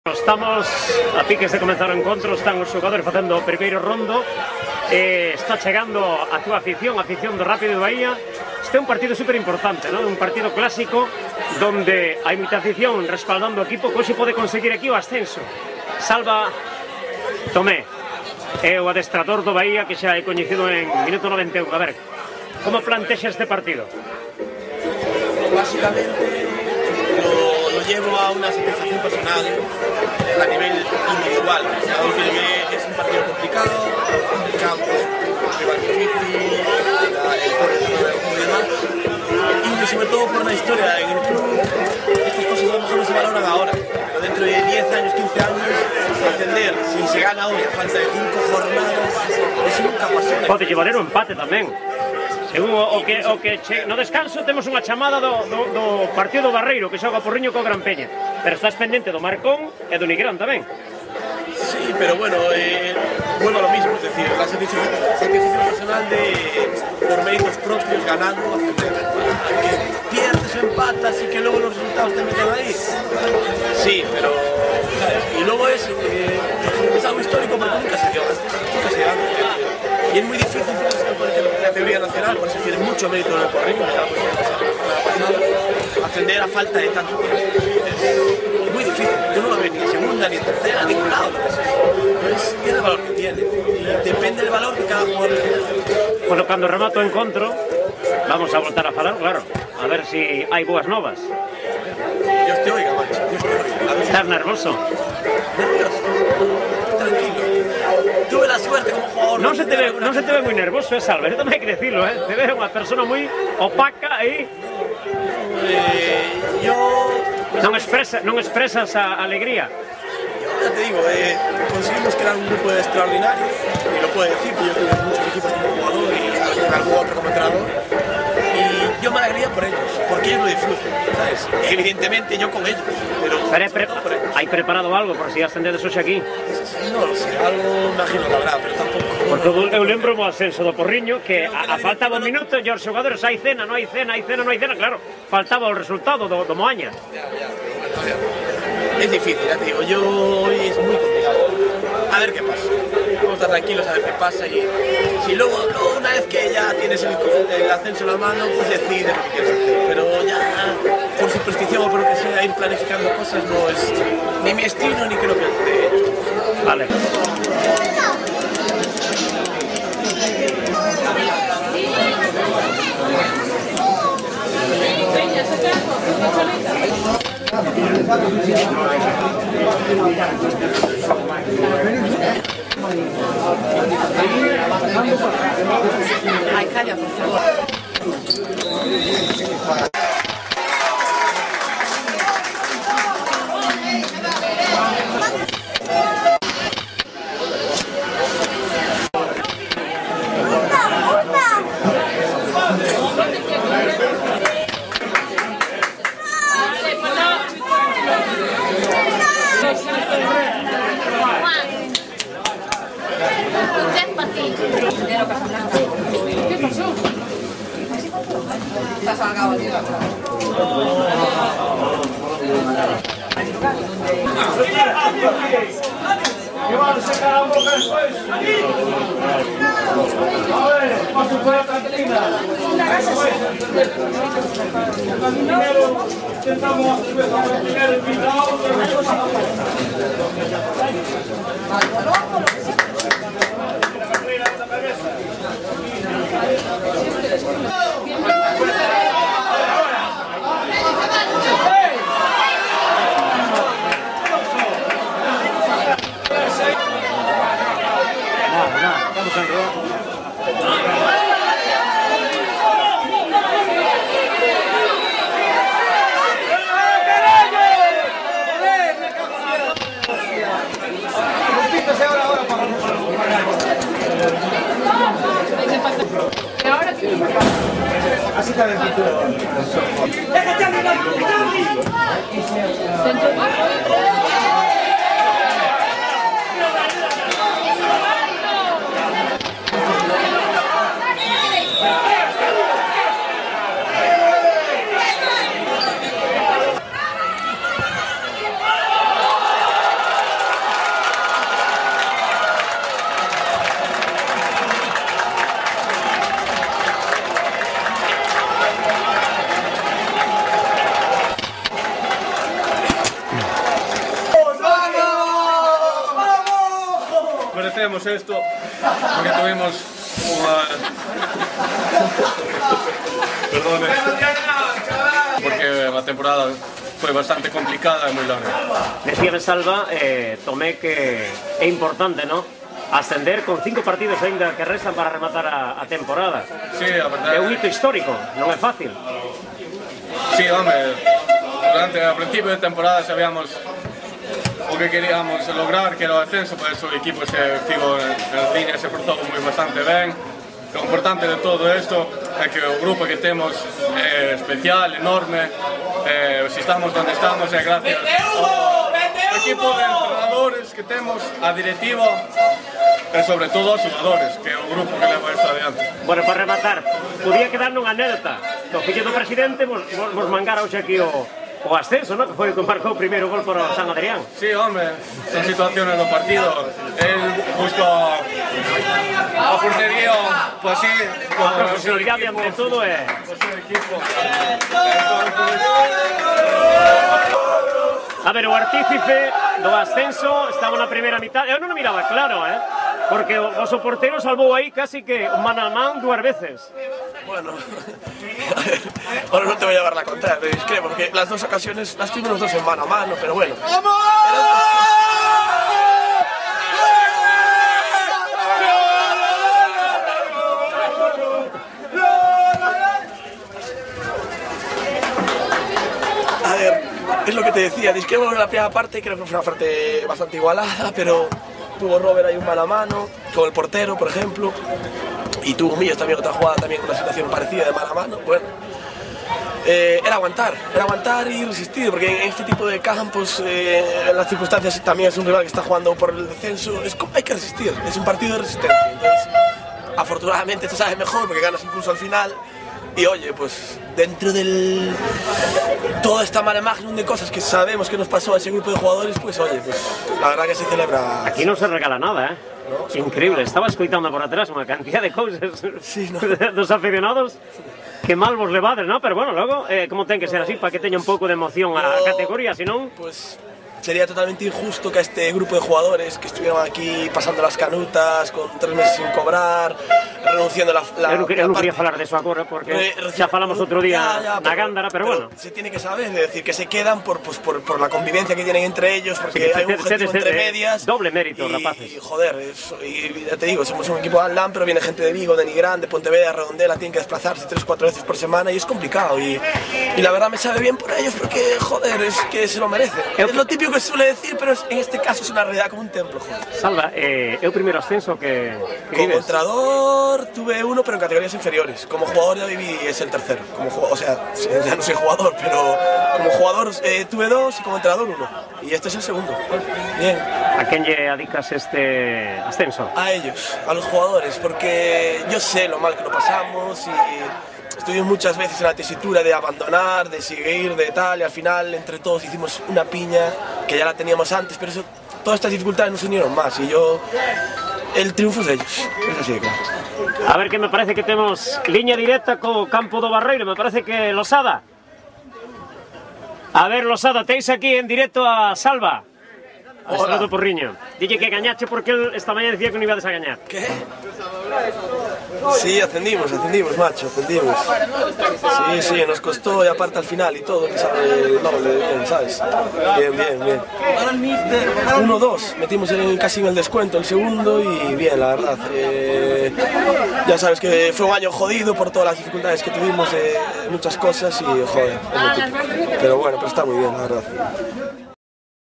MINUTO 91 EN SAN ADRIAN DE COBRES,IMAXENES,ENTREVISTAS E ASCENSO DO BAHIA 2013.
AUDIO DE NEL FERNANDEZ.